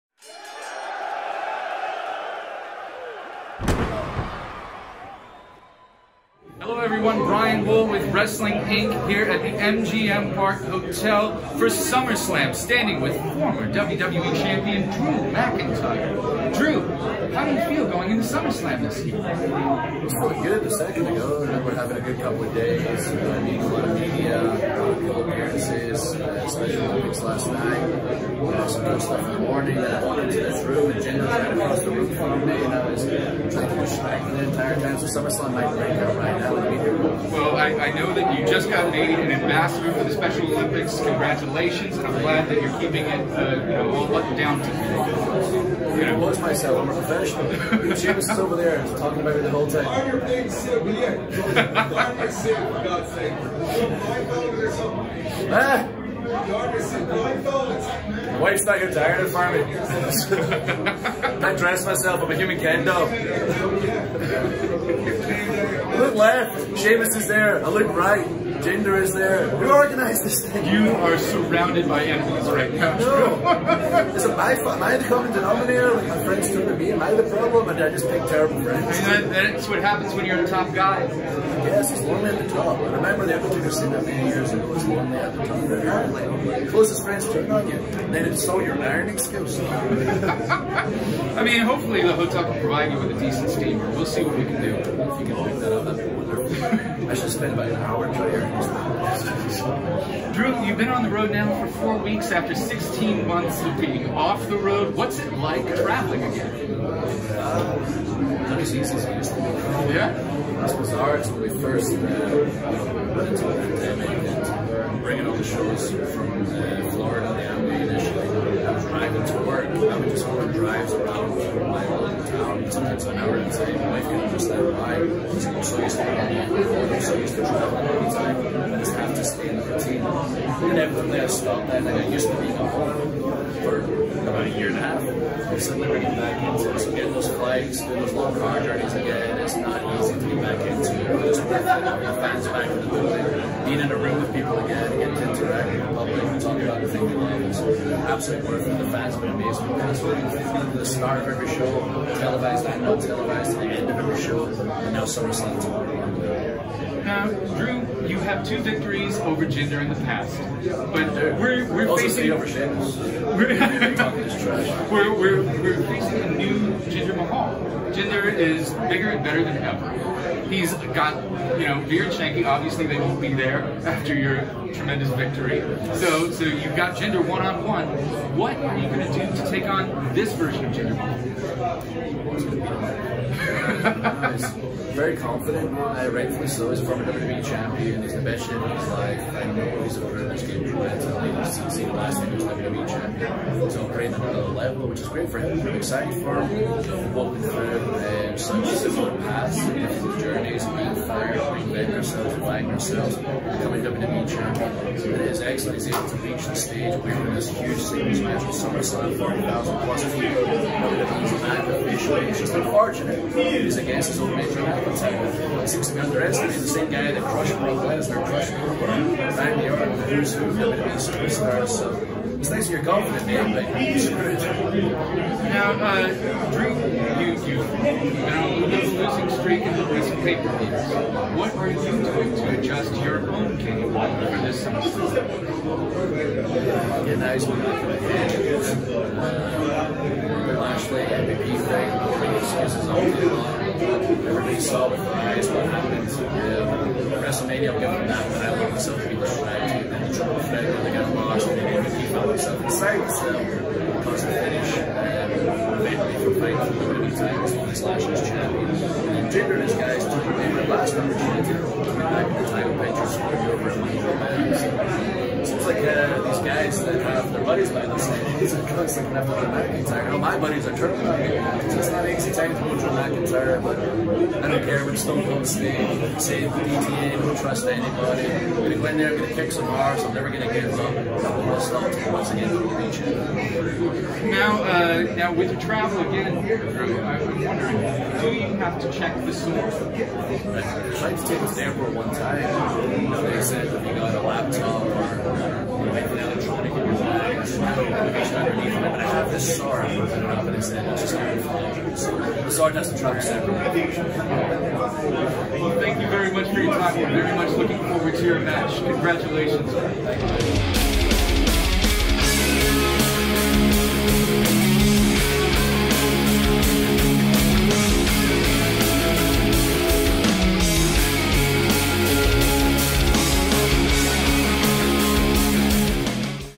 Drew McIntyre Talks Jinder Mahal
Former WWE Champion Drew McIntyre spoke with Brian Wohl of Wrestling Inc. to talk SummerSlam 2021,  Jinder Mahal Being on another level and more!

for more exclusive wrestling interviews and all of the latest news.

For other podcast providers, search "Wrestling Inc. Daily"